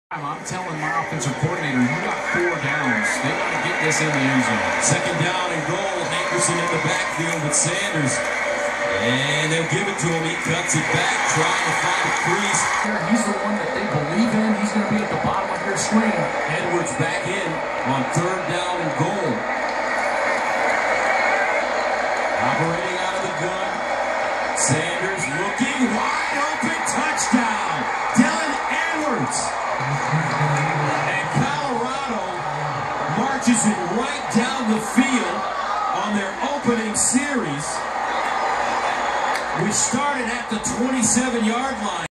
Colorado's first touchdown with Deion Sanders against TCU
first touchdown scored for Colorado with the new head coach deion Sanders versus TCU they do it flawlessly @DEIONSANDERSCOLORADOFAMILY21 @nikefootball @e1coachingcenter @sportsnation @FoxSportsMX